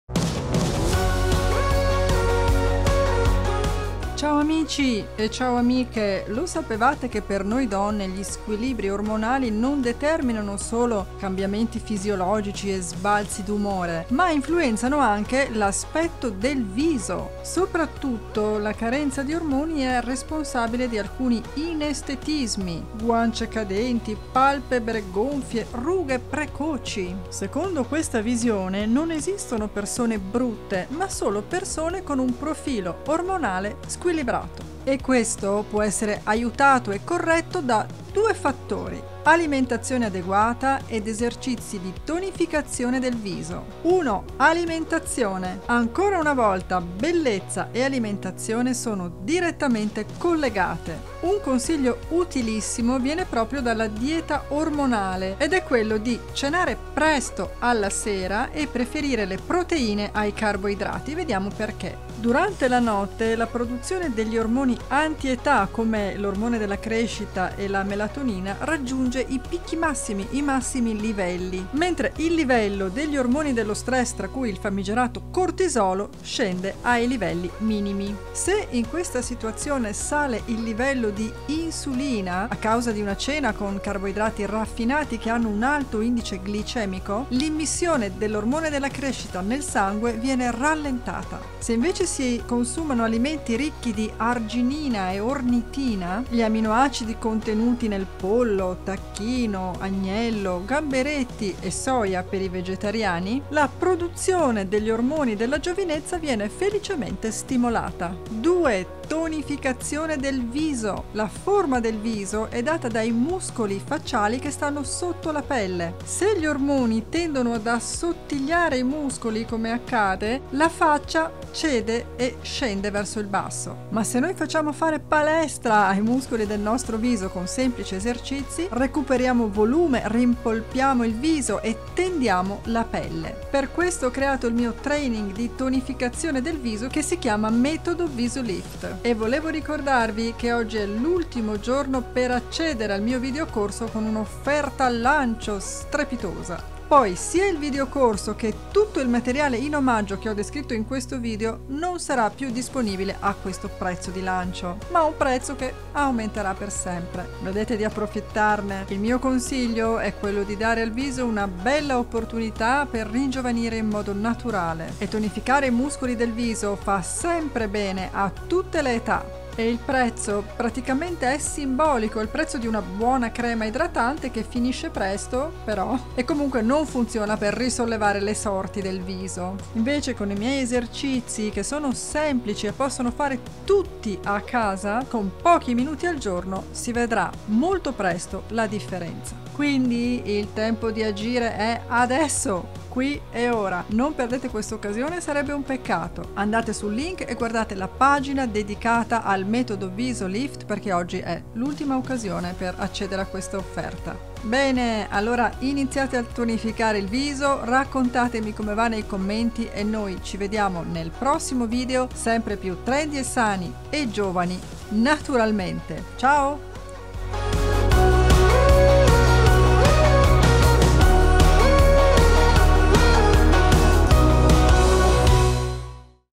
Come AUMENTARE gli ORMONI della GIOVINEZZA con il METODO VISO-LIFT -OFFERTA SPECIALE ancora per poco
GUARDA SUBITO IL METODO VISO-LIFT

Ciao amici! Nel mio ultimo libro dedicato all’Estetica Olistica - che è quasi in stampa e uscirà a maggio - ho descritto il rapporto tra l’aspetto del viso/della pelle e gli ormoni, con alcuni consigli pratici per favorire gli ormoni della giovinezza e contrastare quelli dell’invecchiamento.
E’ molto interessante … Descrivo anche come il mio Metodo Viso Lift di ginnastica del viso sia l’unico metodo naturale che può davvero contrastare l’assottigliamento dei muscoli dovuto all’età e agli ormoni … Come sai il video corso del mio Metodo Viso-Lift è in offerta in questo momento, ma solo fino alla mezzanotte di oggi …

Nota bene

Simona Vignali